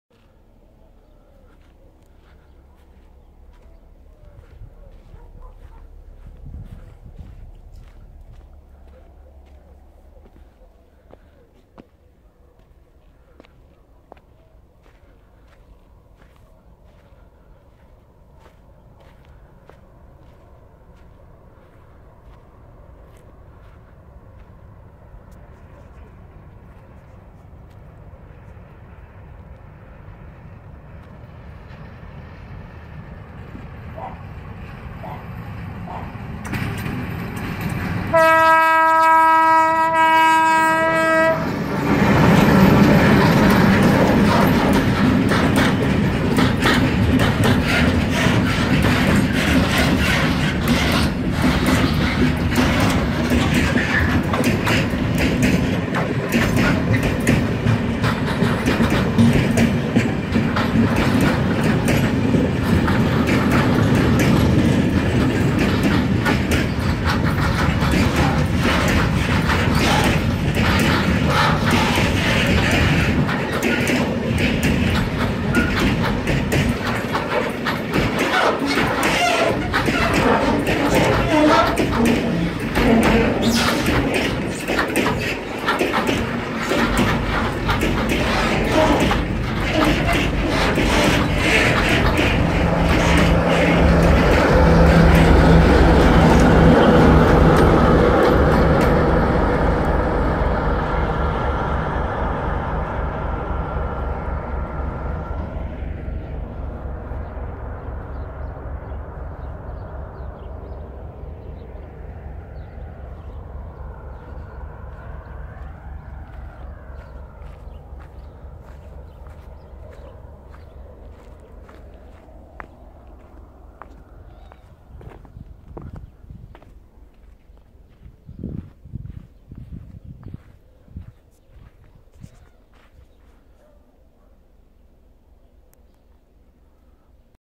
Locomotives SRB-KP 647 006 & 667 100 / Freight train (PA - NS) / Donji Titel - Titel, 28. 03. 2022.
0:00​​​​ Start & ​​Titel & Prolećni dan / ​​Spring day     ​​
0:34 Sirena 1, 2 & Lokomotiva Kombinovani Prevoz SRB - KP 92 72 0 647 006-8  / Locomotive Kombinovani Prevoz SRB - KP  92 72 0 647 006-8
1:40 Kraj voza & Lokomotiva Kombinovani Prevoz SRB - KP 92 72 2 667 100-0   / End of the train  & Locomotive Kombinovani Prevoz SRB - KP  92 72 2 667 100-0
1:50​​ Prolećni dan / ​​Spring day   ​​

Vmax - 20 km/h

Društvo za transport, turizam i trgovinu "Kombinovani prevoz" d.o.o. - Beograd / Company for transport, tourism and trade "Kombinovani prevoz" d.o.o. - Belgrade

Lokomotiva Kombinovani Prevoz (SRB - KP) d. o. o. - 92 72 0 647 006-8 & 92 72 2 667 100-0 / Locomotive Kombinovani Prevoz (SRB - KP) d. o. o. - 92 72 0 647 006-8 & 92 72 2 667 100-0

Alternative line Pančevo Glavna - Novi Sad Ranžirna (via Orlovat Junction) for freight transport while reconstruction is in progress on Corridor X

Prolazak teretnog voza stajalište Donji Titel.

Titel, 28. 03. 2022. godine, u 18.17 h

Napomena: Moj 61. video snimak iz Donjeg Titela u 2022. godini (Titel, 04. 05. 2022.)